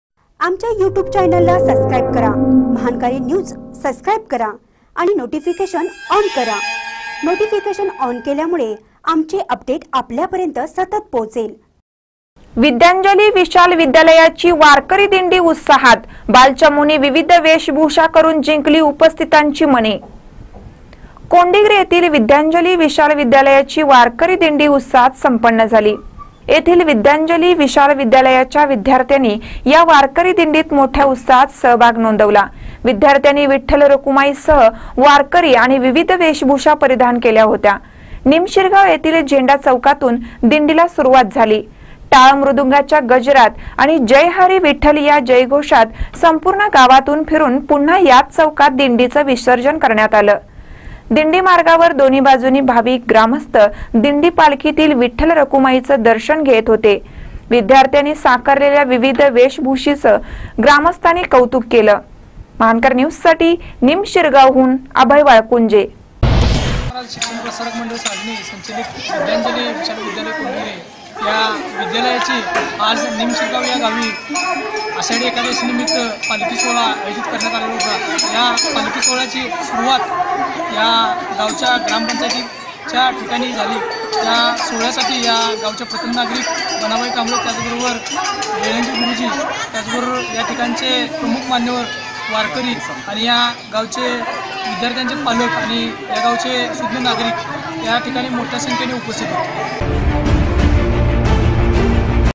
विद्यांजली विशाल विद्यालयाची वारकरी दिंडी उत्साहात
विद्यांजली विशाल विद्यालयाची वारकरी दिंडी उत्साहात : बालचमुंनी विविध वेशभूषा करुन जिंकली उपस्थितांची मने